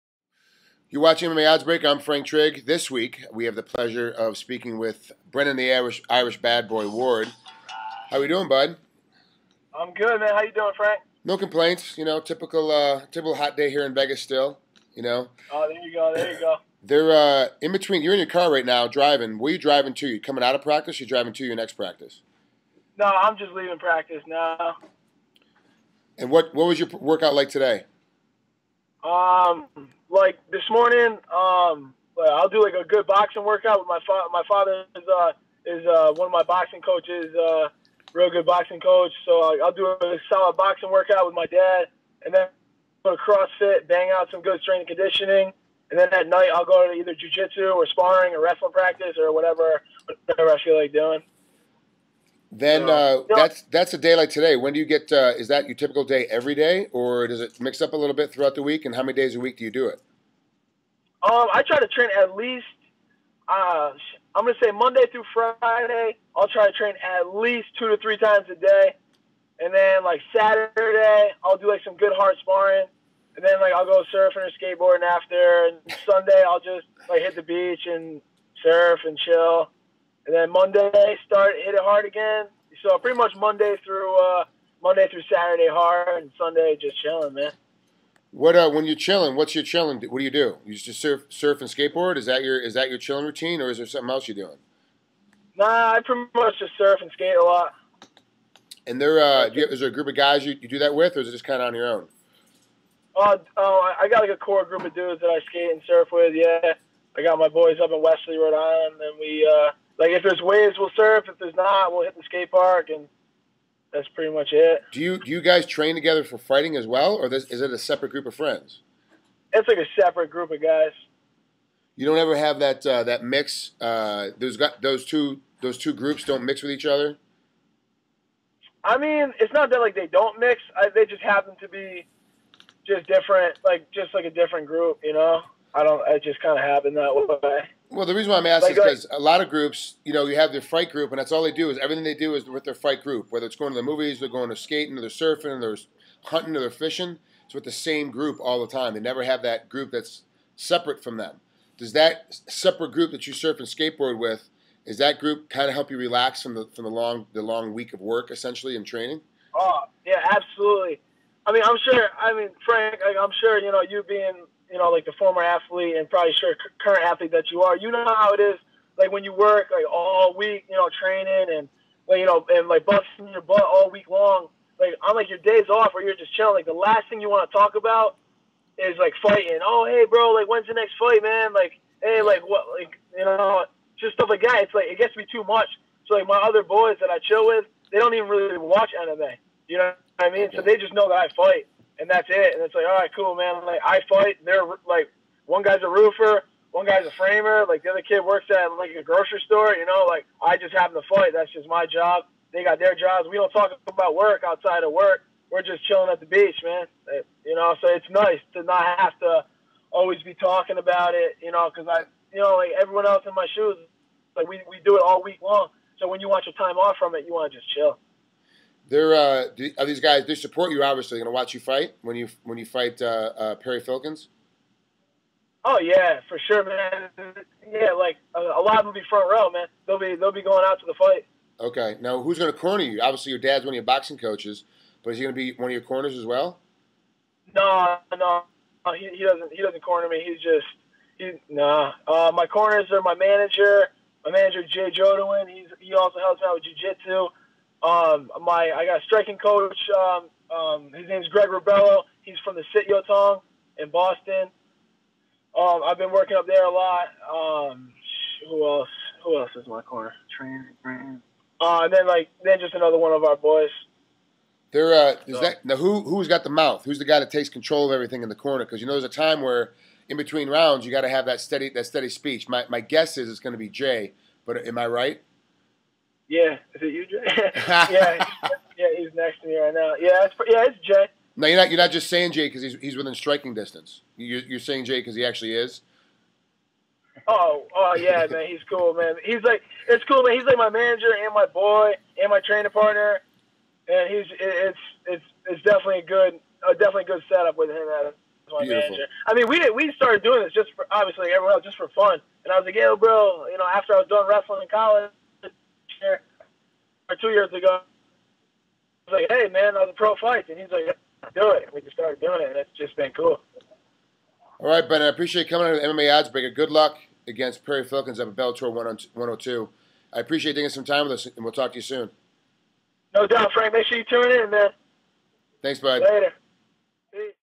Bellator 102 Middleweight Semifinalist Brennan Ward: 'I got into this for fun'
Frank Trigg interviews Brennan Ward before his upcoming middleweight tournament semifinal fight at Bellator 102